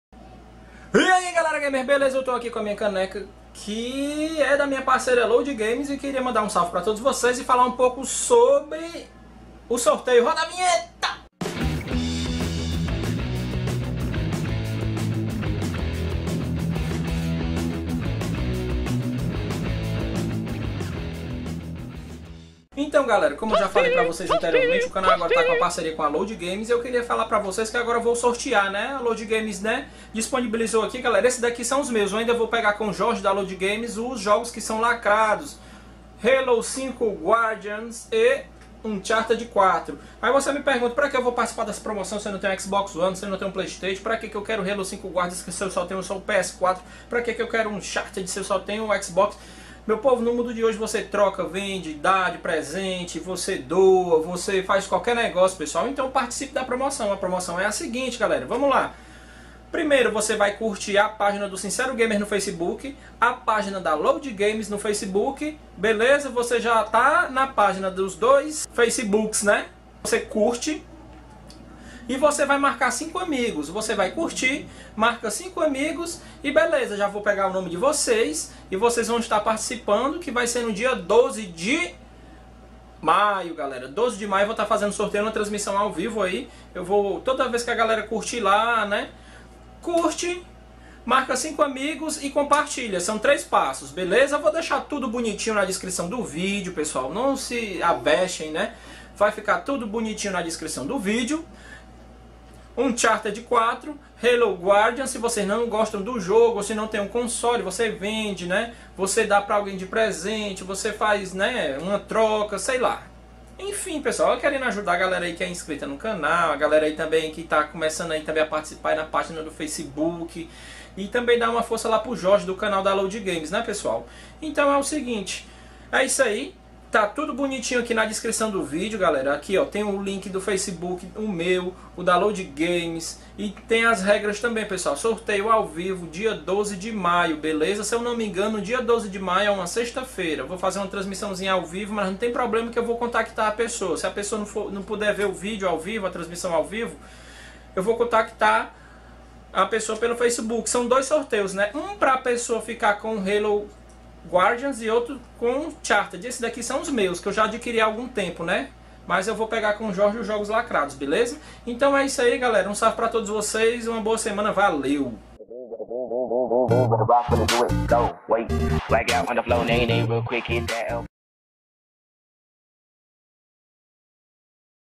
Sorteio - Uncharted 4 & HALO 5 Guardians
FOTO OFICIAL  (SORTEIO 2 JOGOS)
1: CURTE A PAGINA
# JUNIOR SINCERO GAMER
2: MARQUE 5 AMIGOS NA FOTO DO FACE QUE É A OFICIAL DO SORTEIO
3: COMPARTILHE (publico) ESSA POSTAGEM NO SEU FACEBOOK

§§§§§§§§§ sorteio dia 12 de maio §§§§§§§§§§§
Caso  não esteja assistindo ao vivo mesmo assim contactarei no face os 2 ganhadores de HALO 5 E U4
### SE OS 2 SORTEADOS AO VIVO NAO TIVEREM FEITO OS 3 PASSOS ACIMA SORTEAREI OUTRO